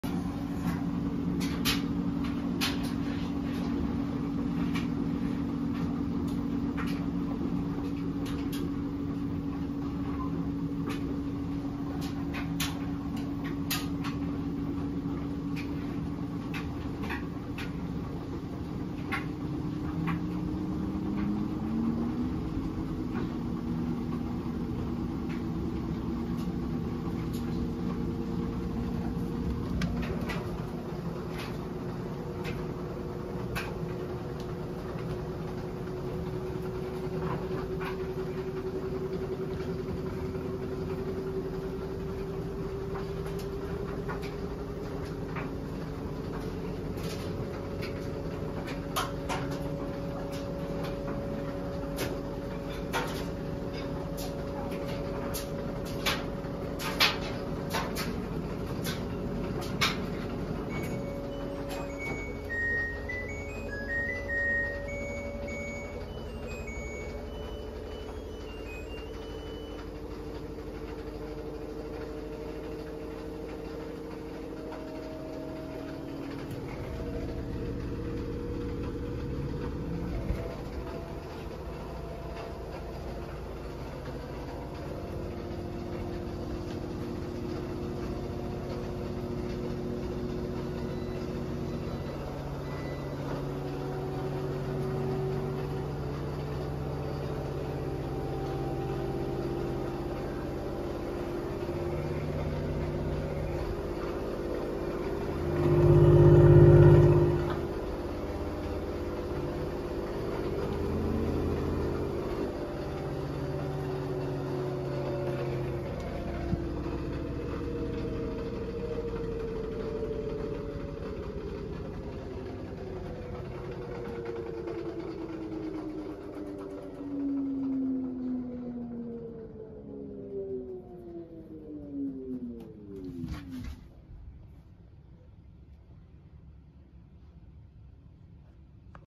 My LG WM3400CW’s Intermittent Spin (Dryer got 1st Place)
@MY PORTFOLIO @lgwashingmachine7835

1100 RPM on an Intermittent Spin